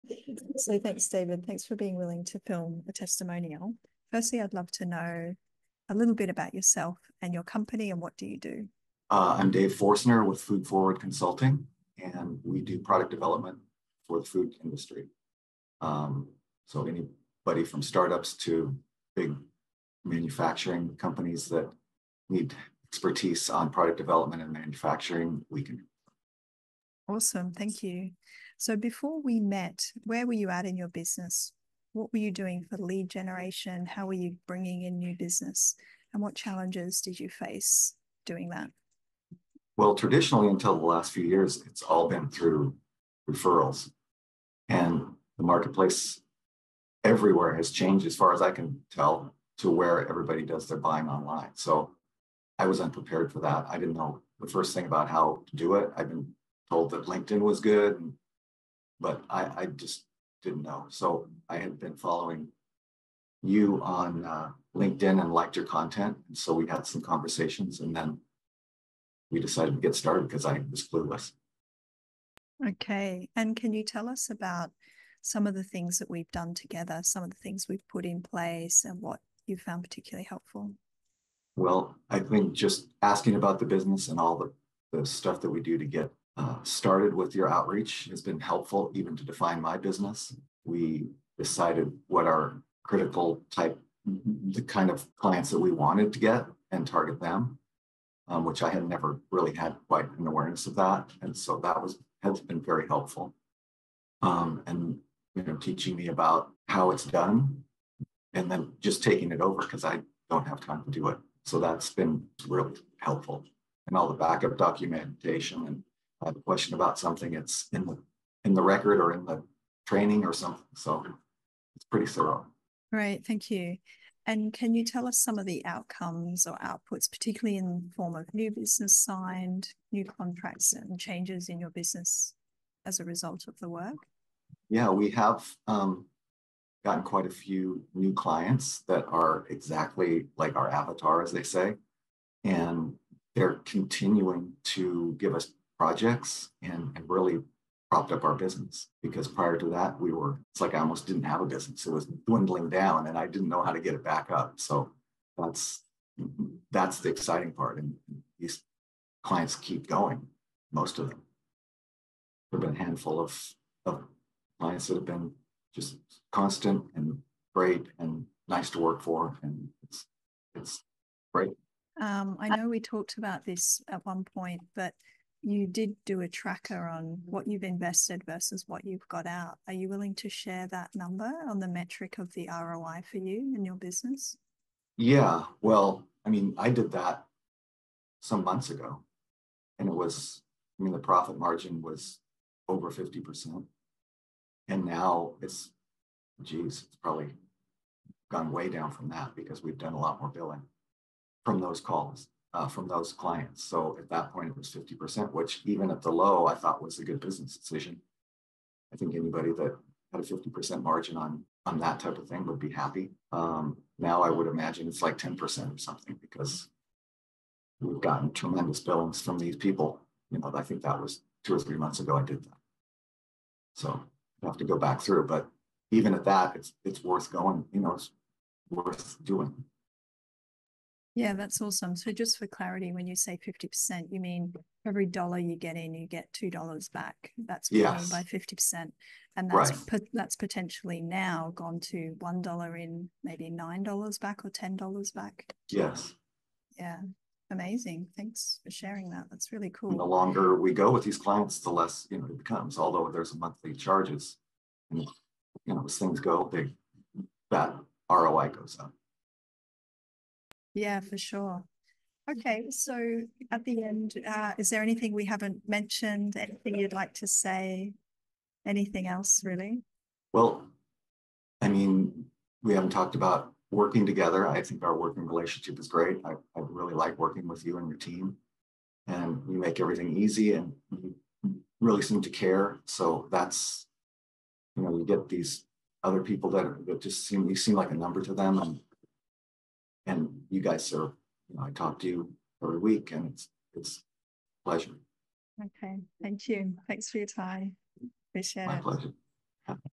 Food Forward Went From 50% Profit to 90% Profit Margin with LinkedIn & Cold Email
David Foerstner the CEO and founder of Food Forward, a boutique consultancy helping food and beverage manufacturers to develop and bring new products to from concept to launch. He went from relying on word of mouth and a declining business to gaining up to 90% profit margins by landing great clients that are creating recurring income for him.

To find out how your business can grow your new business pipeline using LinkedIn and cold email check out or email